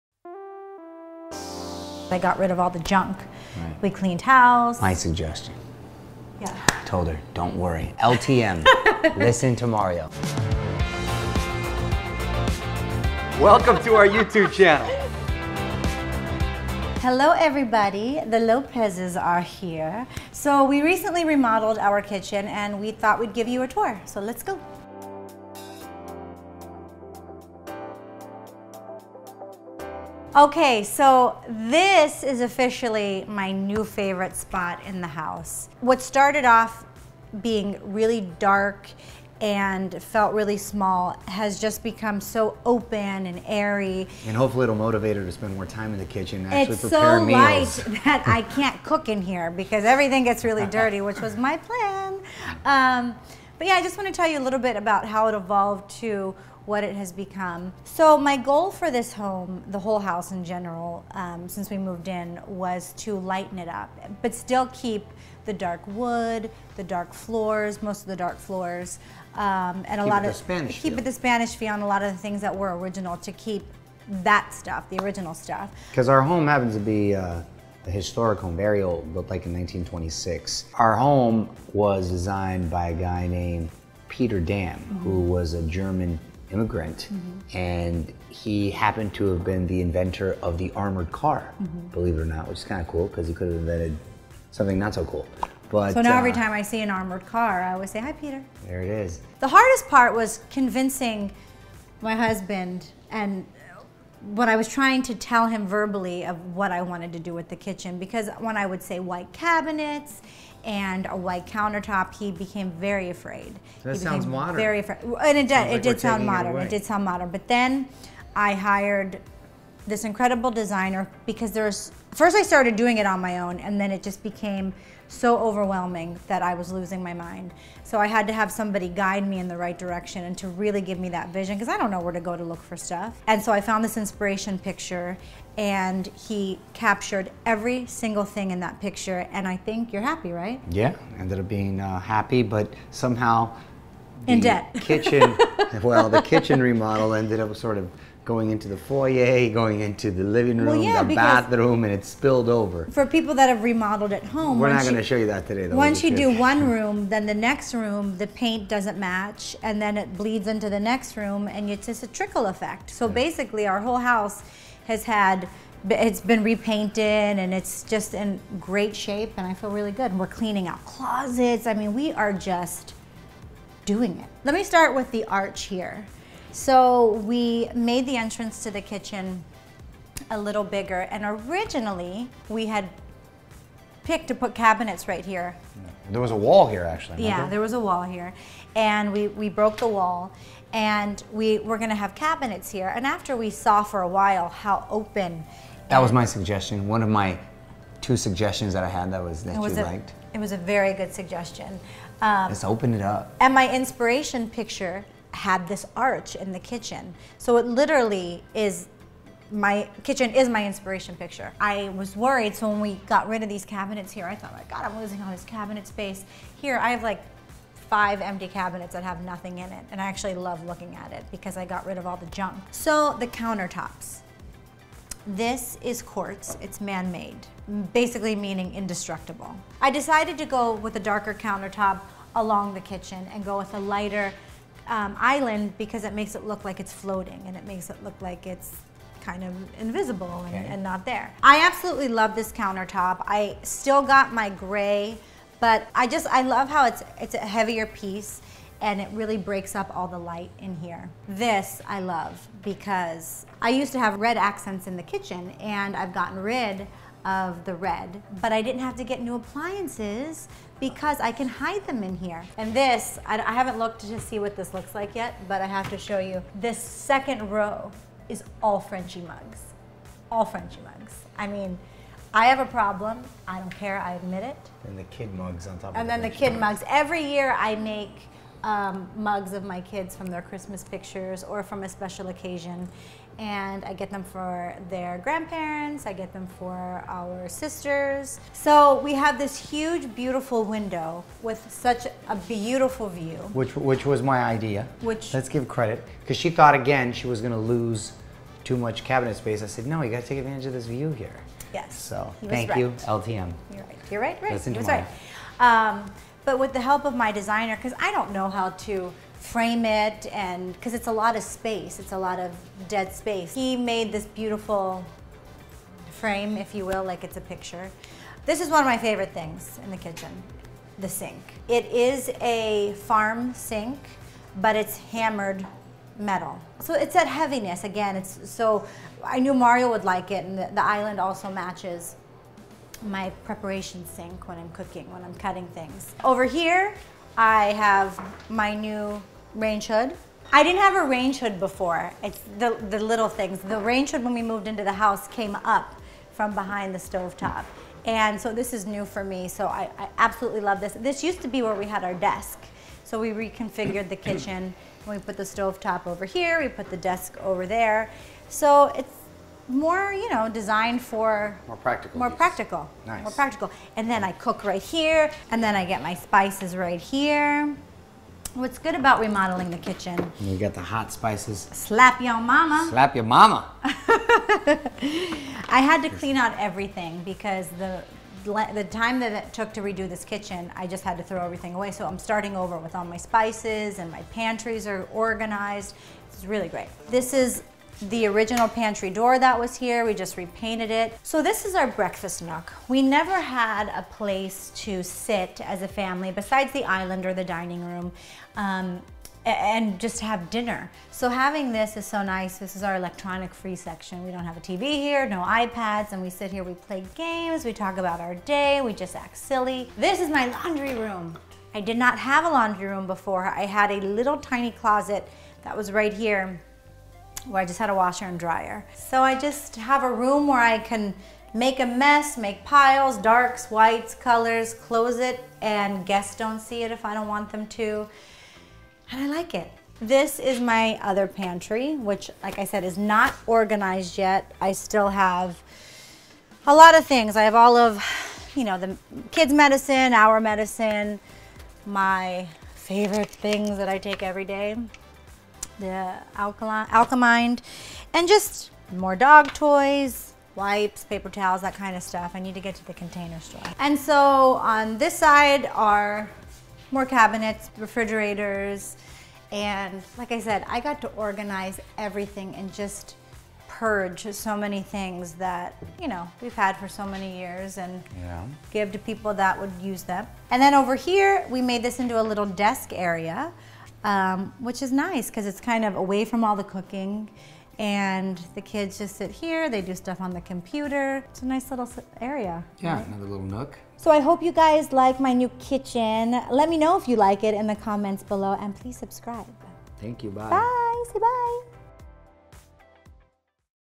Courtney and Mario Lopez Kitchen Remodel and Tour
Courtney and Mario Lopez renovated their kitchen! Check out the transformation and hear what Courtney and Mario love most about the new space.